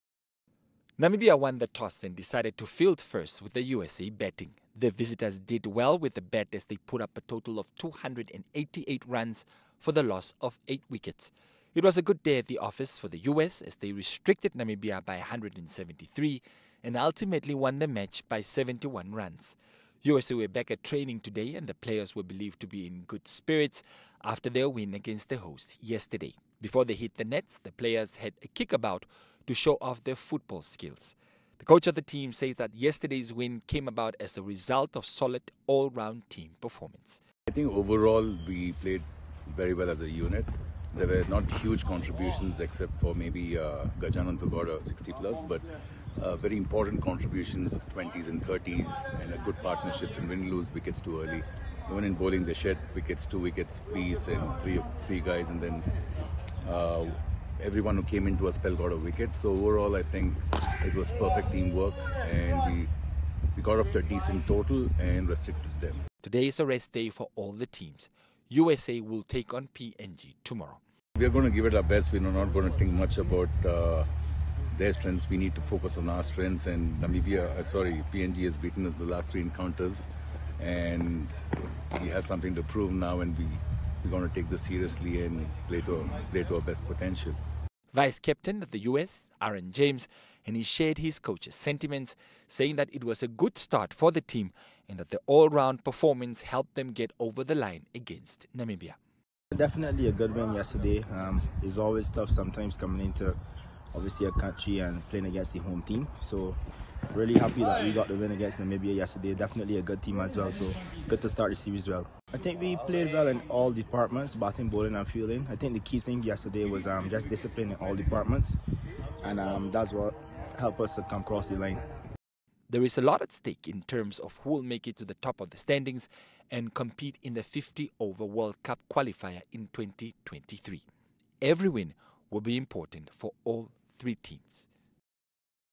USA BEAT NAM
The coach of the visiting USA cricket team, J Arun Kumar, says he was very happy with his side's 71 runs win in their opening match of the ICC Men's Cricket World Cup League 2 series, which took place at Wanderers Cricket Ground on Monday.